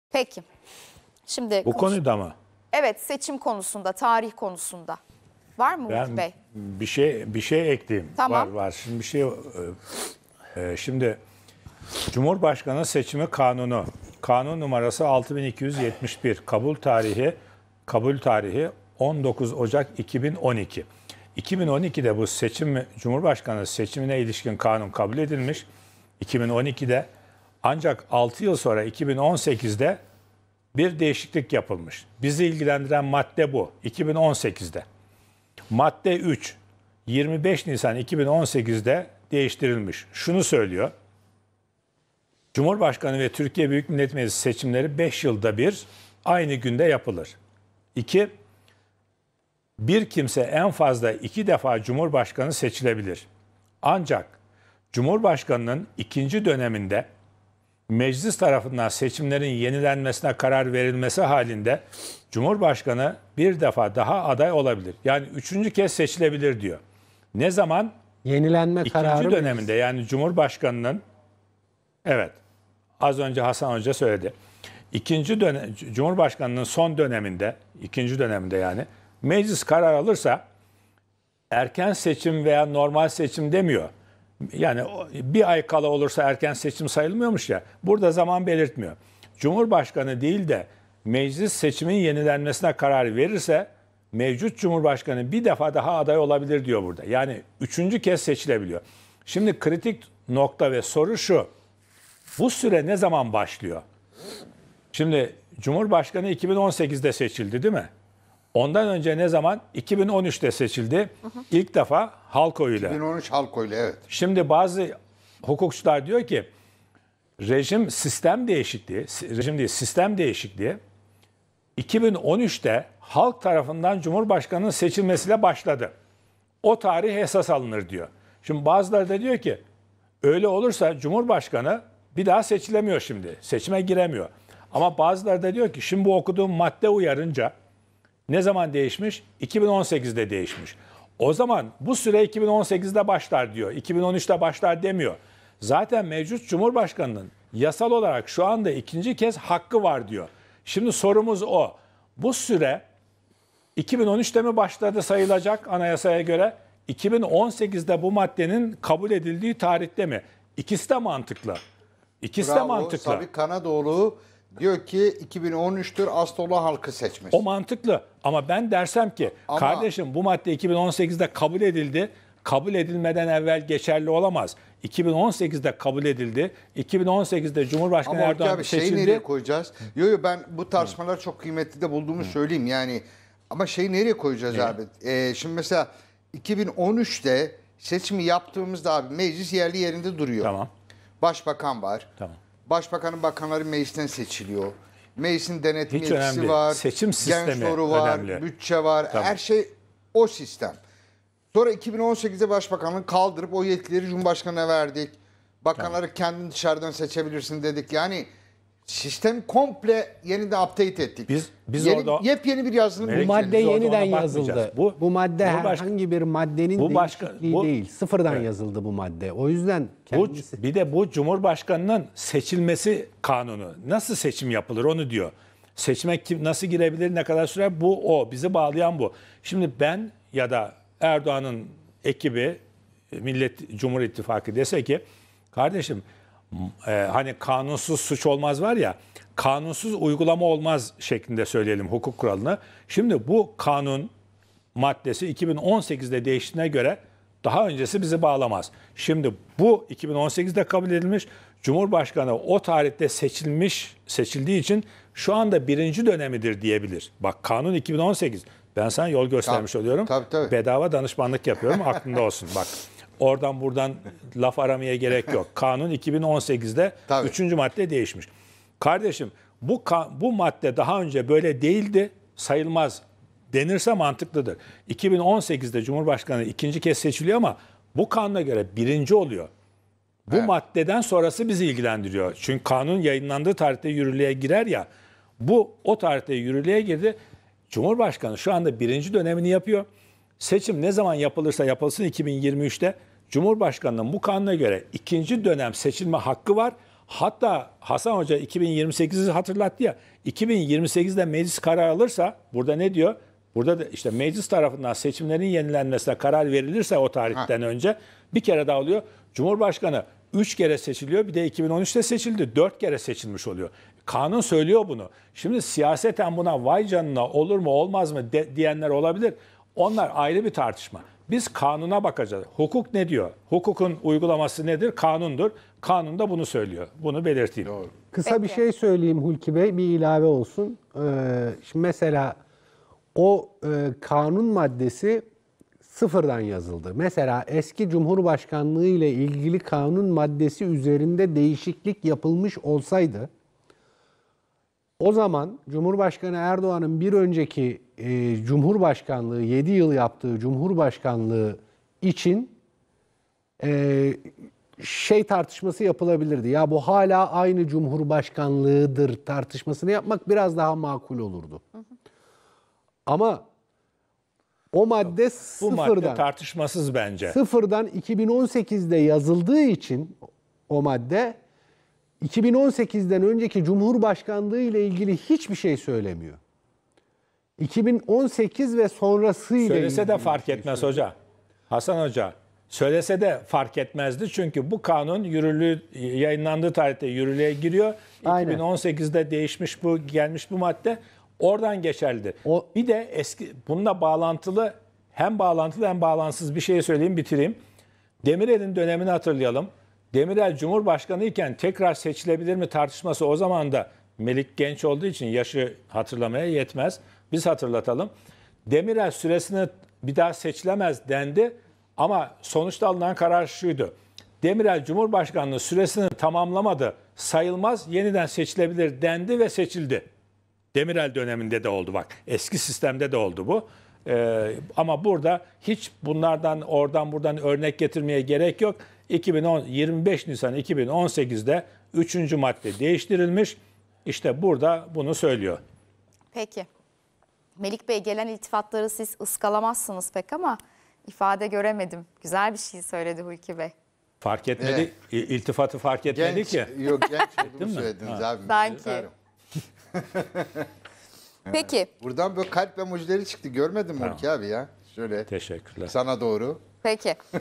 Seçim kararında süreç nasıl işliyor? Seçim kararını kim alacak? Hulki Cevizoğlu yorumladı
Siyasetin gündemine dair her şey Akıl Çemberi’nde değerlendiriliyor. Memur ve emekliye yüzde 25 zam! Gözler orta gelirliye konut projesinde! Seçim kararında süreç nasıl işliyor? Seçim kararını Meclis mi alacak, cumhurbaşkanı mı? Seçimler "14 Mayıs'ta" mı yapılacak? Deniz Baykal o fotoğraftan mutlu mu? "Lozan bitti" yalanını kimler uydurdu? Kılıçdaroğlu 2 gün sonra aday mı? Semiha Şahin sordu, Sosyolog-Gazeteci Doç.Dr. Hulki Cevizoğlu, Siyaset Bilimci Prof.Dr. Hasan Basri Yalçın, Güvenlik Politikaları Uzmanı Mete Yarar ve Gazeteci Melik Yiğitel yanıtladı.